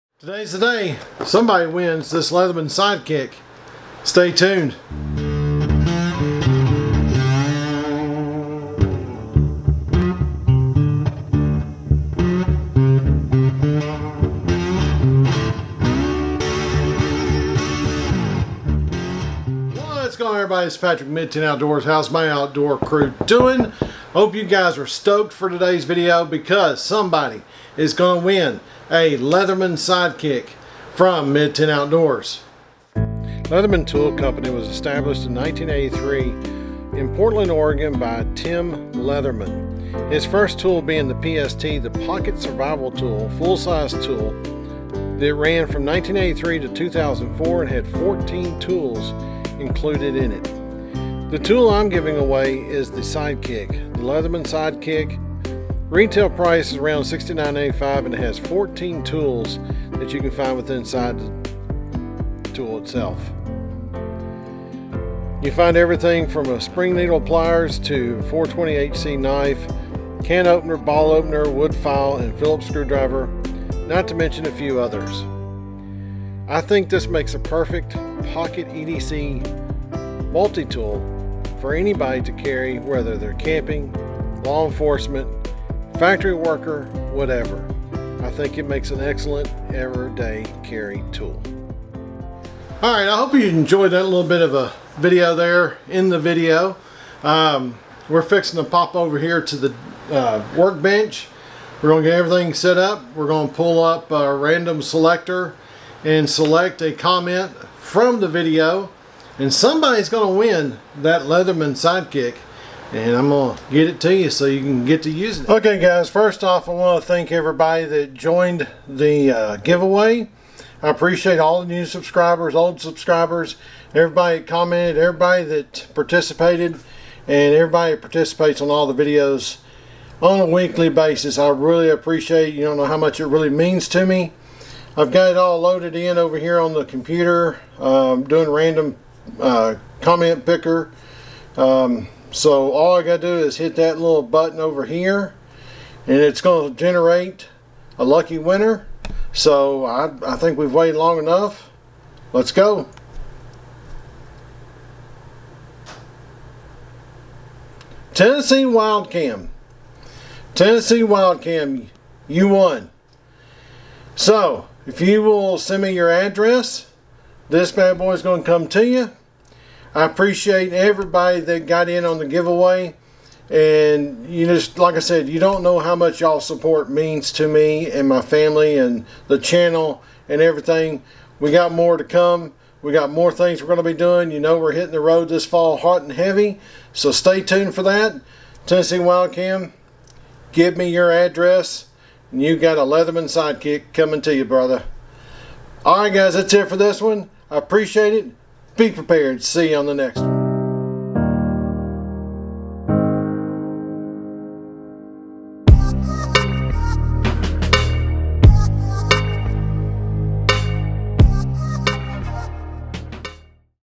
Leatherman Sidekick Winner IS
In this video, we pick the lucky winner of the Leatherman Sidekick.